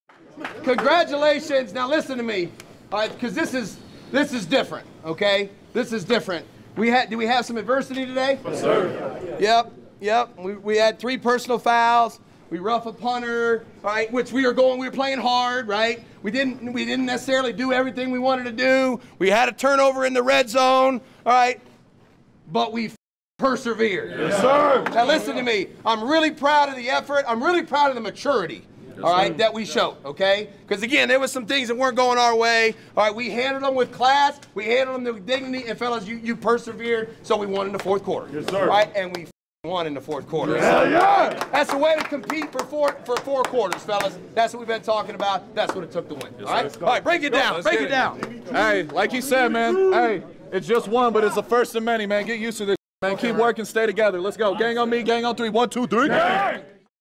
Josh McDaniels’ Locker Room Victory Speech vs. Broncos: ‘That’s the Way to Compete for 4 Quarters'
Go inside the locker room for Head Coach Josh McDaniels' postgame victory speech following the Raiders' 17-16 win over the Denver Broncos in the season opener of the 2023 season.